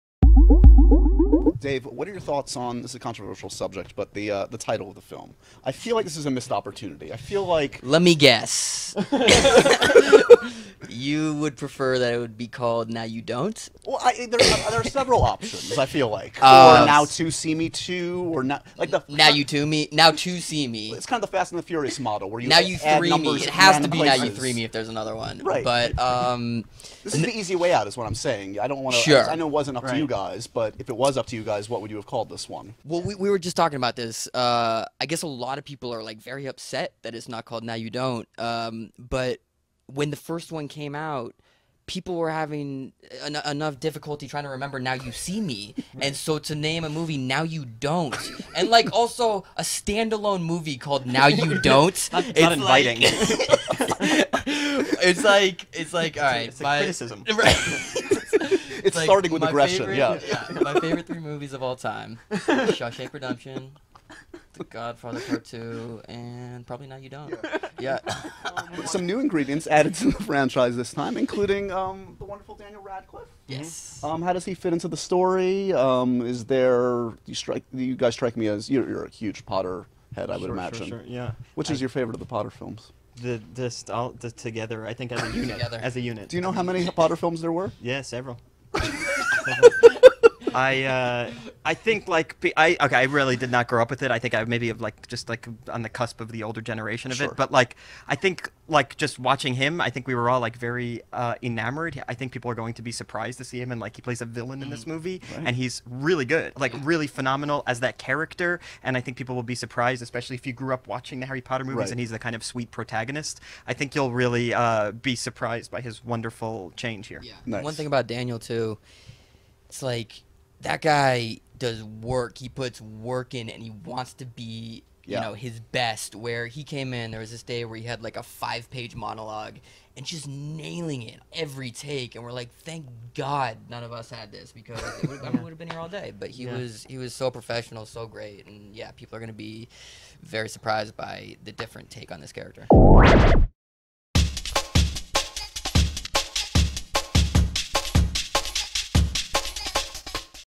Jesse Eisenberg & Dave Franco Pitch New Titles for 'Now You See Me 2' | MTV News
At CinemaCon 2016, MTV New's Josh Horowitz offers advice for how Jesse Eisenberg and Dave Franco can improve the title of their new movie Now You See Me 2, and the actors praise Daniel Radcliffe's villainous performance in the film.